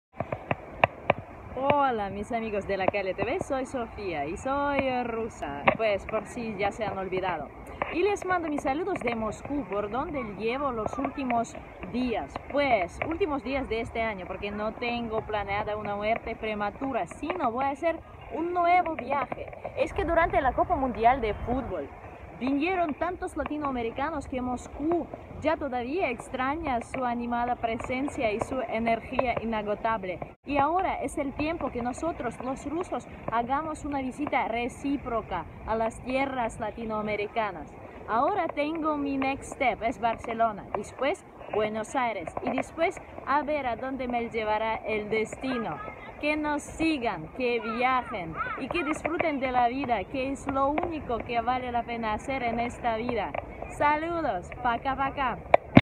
Unos días antes de un nuevo viaje_18_10_2018
Si les ha pasado que cono si dentro tuvieran esta sensación inexplicable, como muy fuertes ganas de viajar, atreverse, meterse en unas aventuras impredecibles?
Conmigo pasa. Regularmente y ya de nuevo no puedo superar esta ansia y me voy...